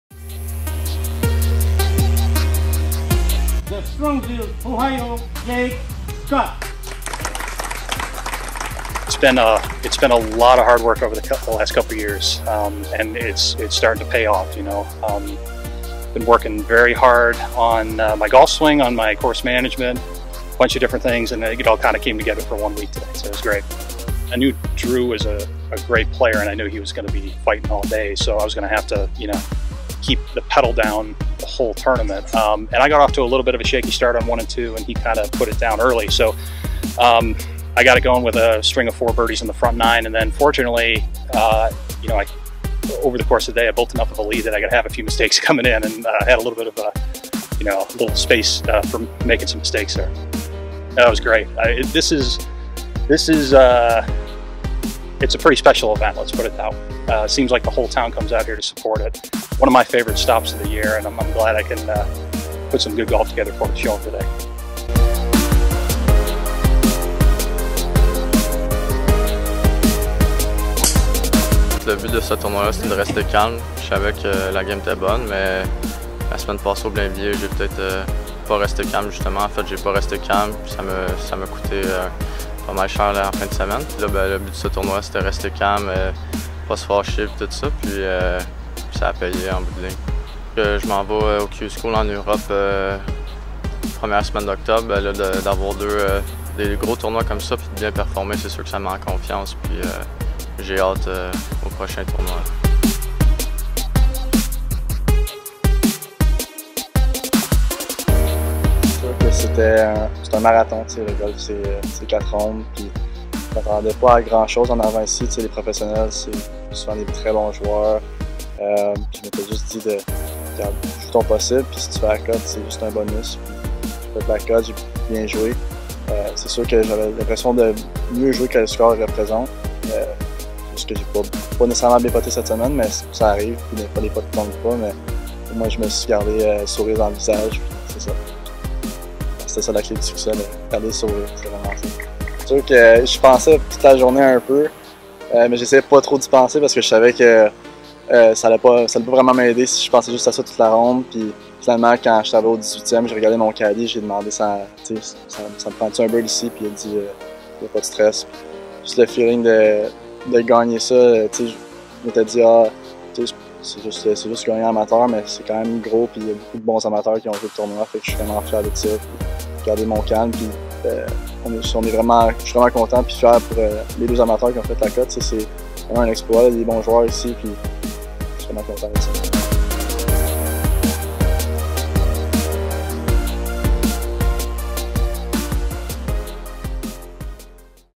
COUPE CANADA 2022
L’inébranlable Jake Scott triomphe à Victoriaville!

L’Américain s’est assuré de conclure son tournoi en beauté pour repartir avec les grands honneurs de la Coupe Canada Sani-Marc – Fenergic, présentée par la Ville de Victoriaville!👏

Brandon Rattray (-2) remporte le titre chez les amateurs!⛳

Sportcom était sur place pour recueillir leurs commentaires!